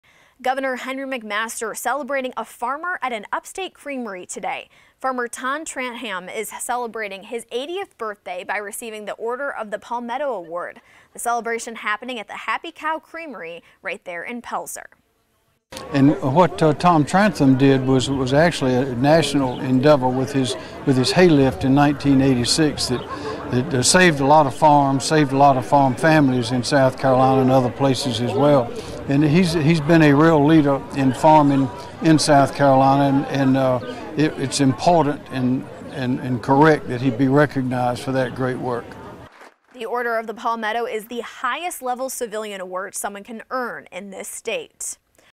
Pelzer man receives Order of the Palmetto Award
The award is the highest civilian honor in the state of South Carolina.